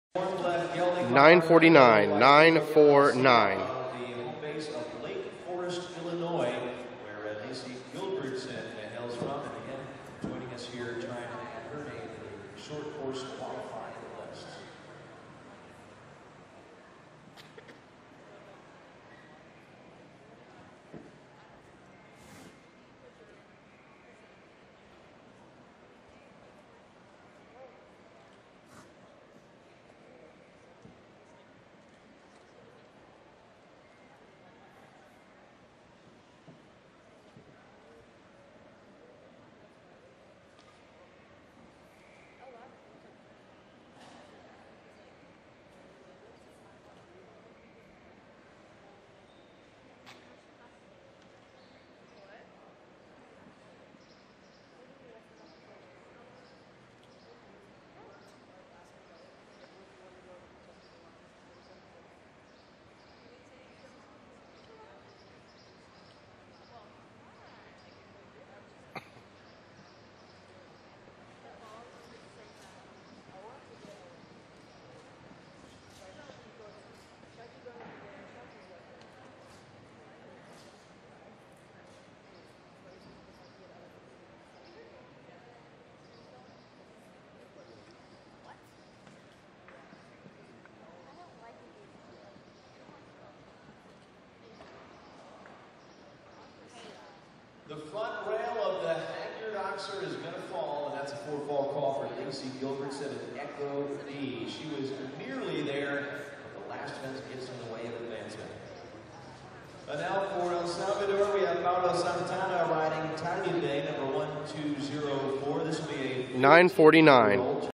Video of ECHO D ridden by LACEY GILBERTSON from ShowNet!
ECHO D ridden by LACEY GILBERTSON at Kentucky Spring I 2015.  This round is from class 450, HAGYARD LEXINGTON CLASSIC CSI2* II 2a and finished 29 of 74.